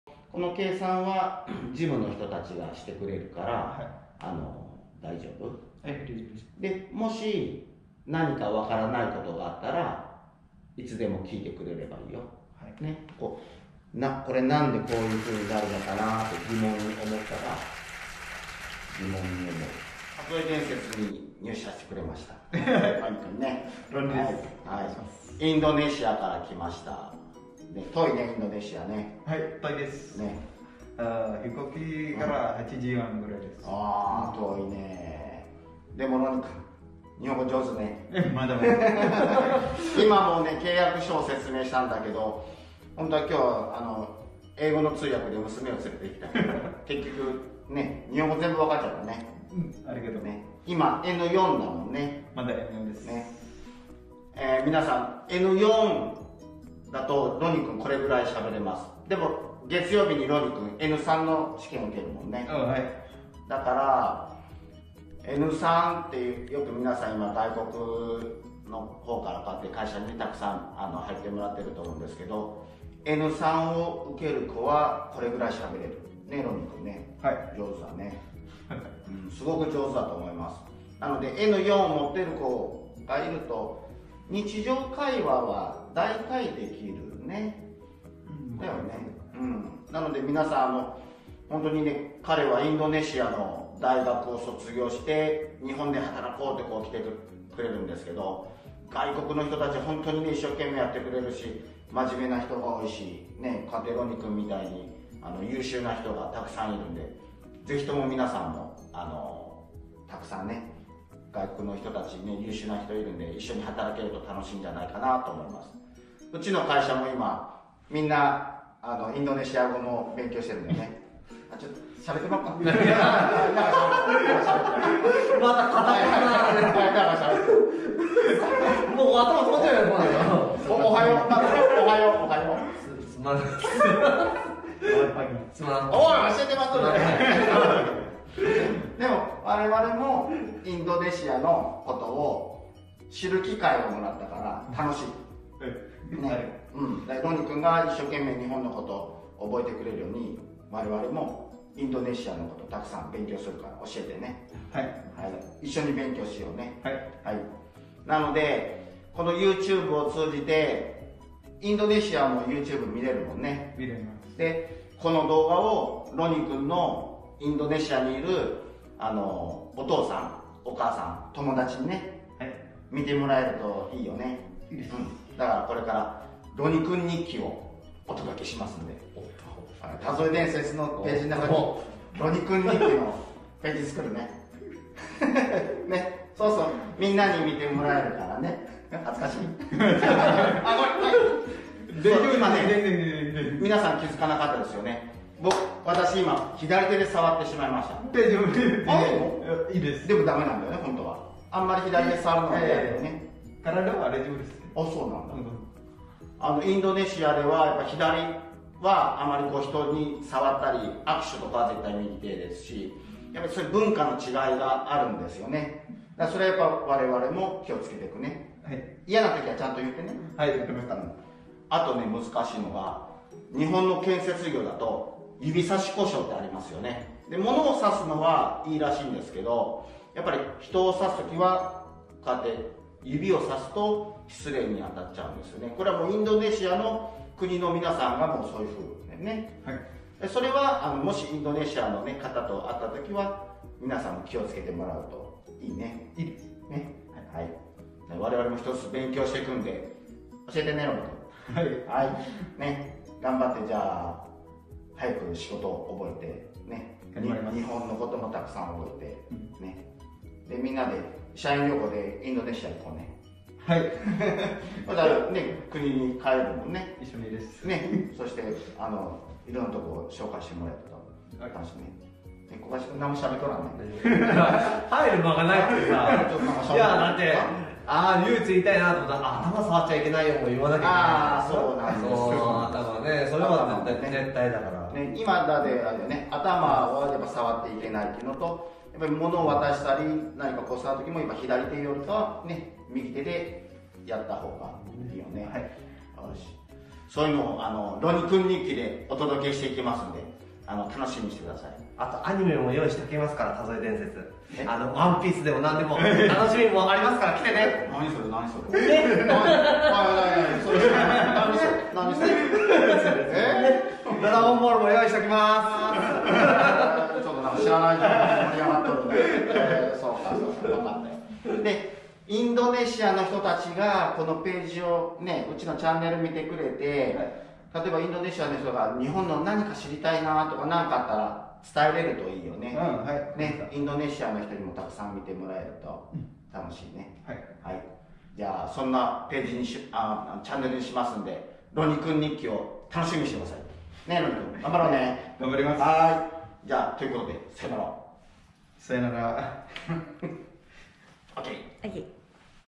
【田副電設】インドネシア出身のロニ君が入社してくれました！めちゃ日本語上手。
新入社員のロニ君です。インドネシアの大学を卒業している優秀な子です。これからインドネシアの方からの質問にも答えていきます。

▼タゾエグループチャンネル　登録お願いします！